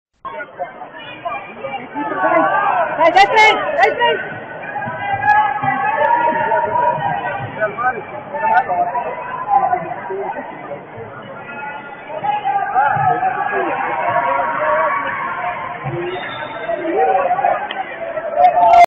Torneo Basket Trento 2012
Finale Torneo Mini Basket Trento Aquilotti 2001.
1° tempo vince Ca' Ossi Forlì 'Neri' vs Arcobaleno Trento 'Blue'.
Finale partita Ca' Ossi Forlì batte Arcobaleno Trento 4 tempi a 2.